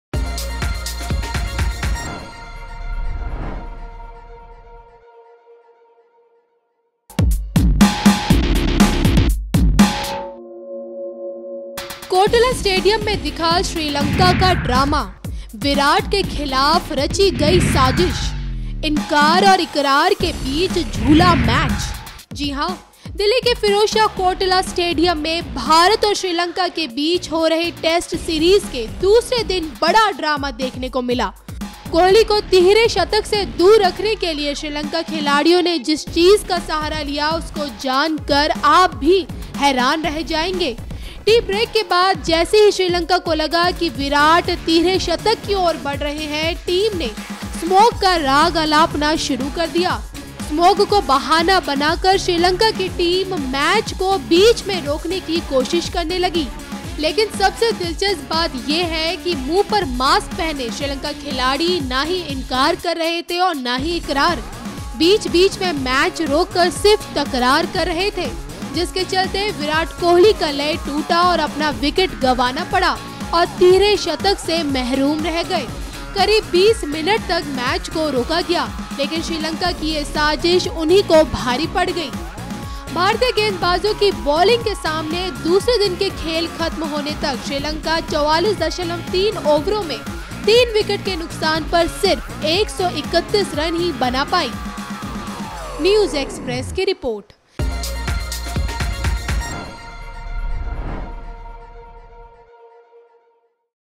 IND VS SL 3rd Test Match : Virat Kohli को रोकने के लिए Sri Lanka ने लिया Fog का सहारा
IND VS SL 3rd Test Match : In this video we tell you that Sri Lanka players complain pollution in between the match.
कोटना स्टेडियम में दिखा श्रीलंका का ड्राम... विराट के खिलाफ रची बड़ी साजिश. इनकार और इकरार के बीच झूला मैच। जी हां...दिल्ली के फिरोजशाह कोटला स्टेडियम में भारत और श्रीलंका के बीच हो रहे टेस्ट सीरीज के दूसरे दिन बड़ा ड्रामा देखने को मिला।